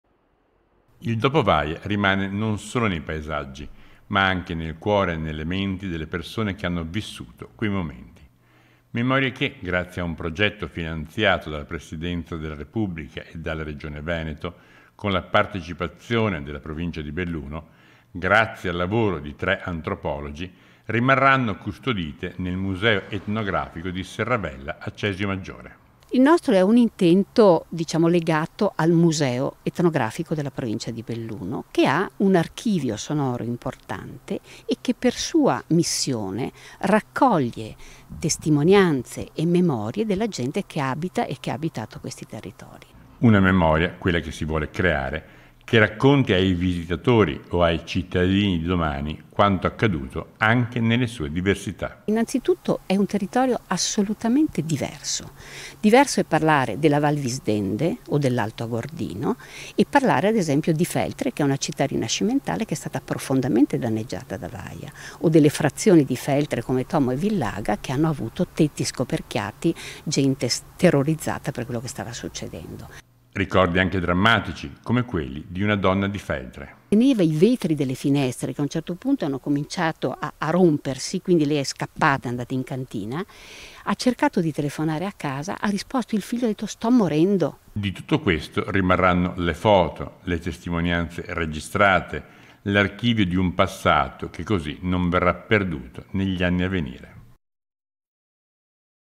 STUDIANDO IL DOPO VAIA | 27/08/2021
A3 NEWS Treviso 27/08/2021 - CESIOMAGGIORE - Daniela Perco, Iolanda De Deppo e Michele Trentini: sono i nomi dei tre antropologi che stanno completando uno studio sul dopo Vaia.  || Daniela Perco, Iolanda De Deppo e Michele Trentini: sono i nomi dei tre antropologi che stanno completando uno studio sul dopo Vaia.che non rimane solo nei paesaggi, ma anche nel cuore e nelle menti delle persone che hanno vissuto quei momenti.Memorie che, grazie a un progetto finanziato dalla Presidenza della Repubblica e dalla Regione Veneto, con la partecipazione della Provincia di Belluno, grazie al lavoro di tre antropologi, rimarranno custodite nel museo etnogrtafico di Serravella a Cesio Maggiore.Una memoria, quella che si vuole creare, che racconti ai visitatori o ai cittadini di domani quanto accaduto anche nelle sue diversità.Di tutto questo rimarranno le foto, le testimonianze registrate, l'archivio di un passato che così non verrà perduto negli anni a venire. - Intervistati DANIELA PERCO (ANTROPOLOGA) (Servizio di Fabio Fioravanzi) - Segui Antenna Tre anche sul digitale terrestre! Visita il sito